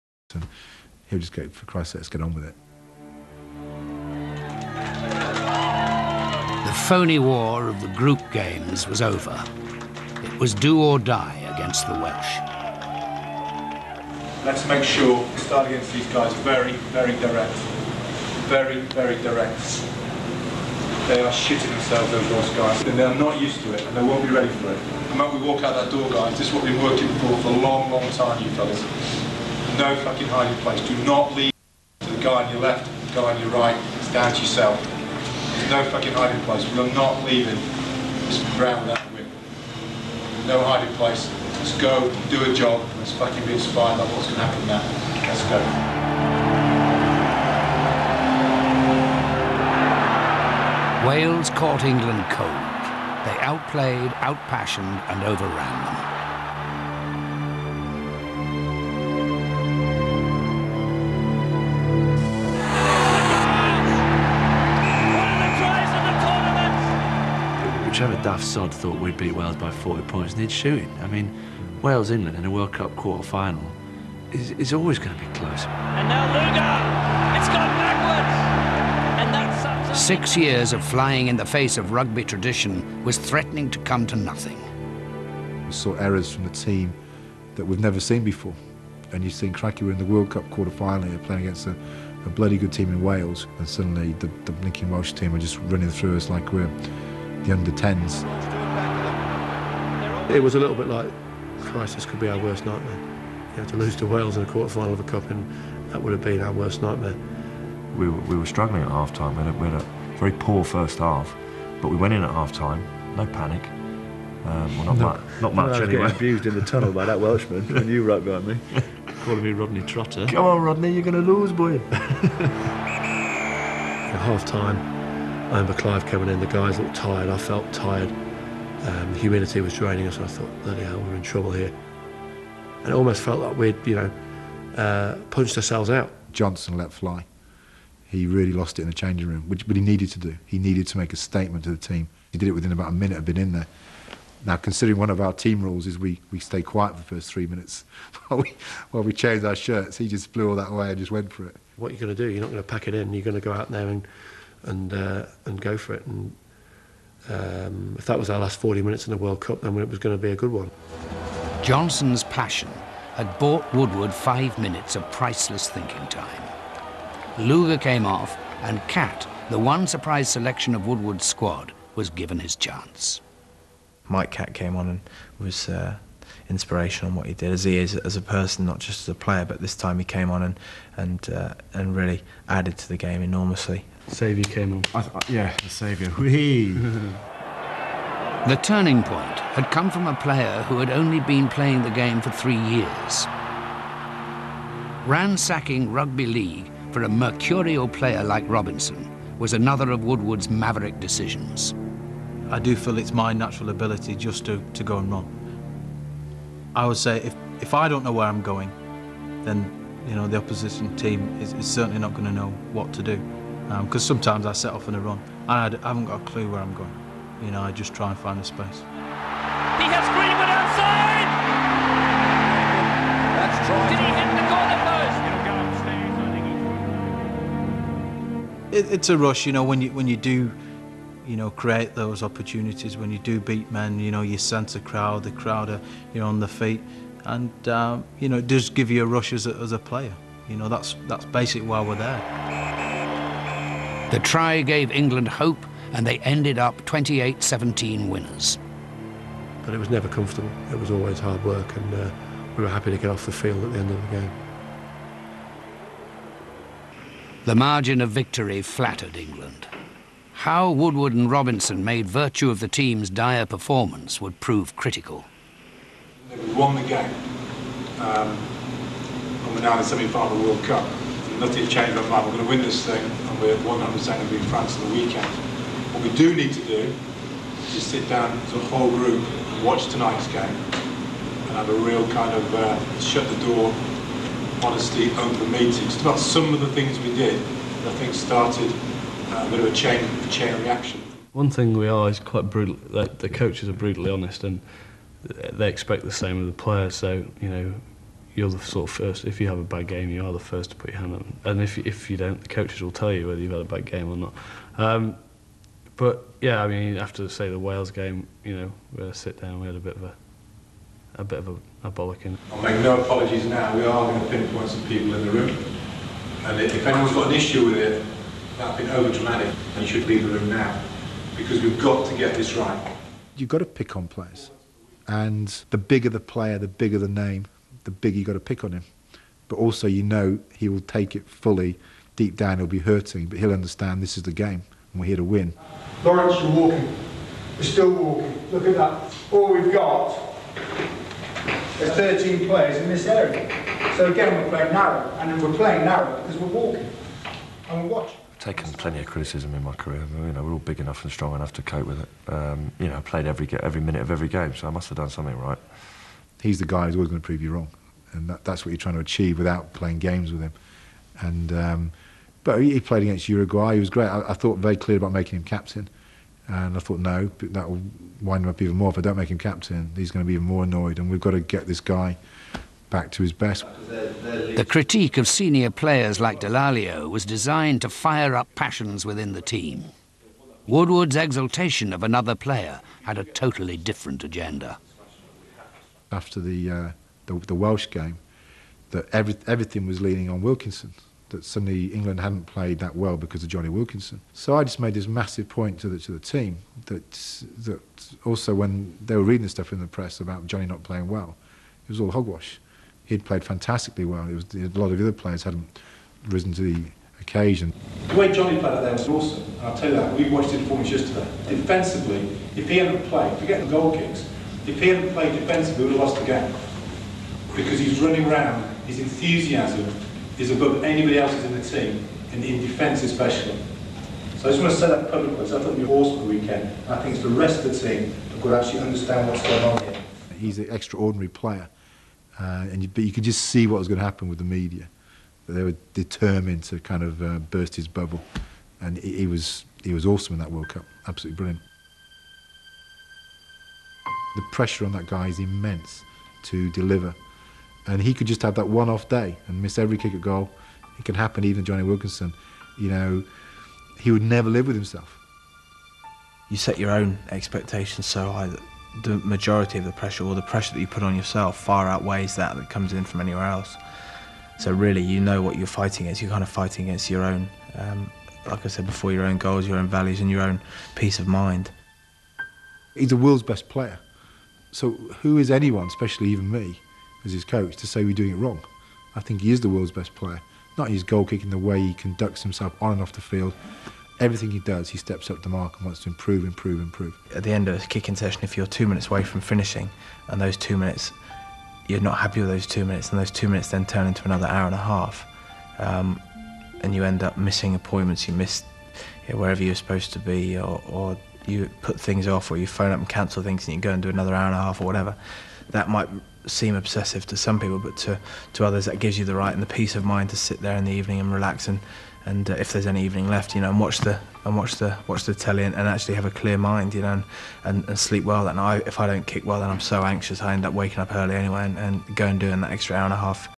Clive Woodward RWC 2003 Part 3
Part 3 of a behind the scenes look at England's World Cup triumph in 2003. Some great insights.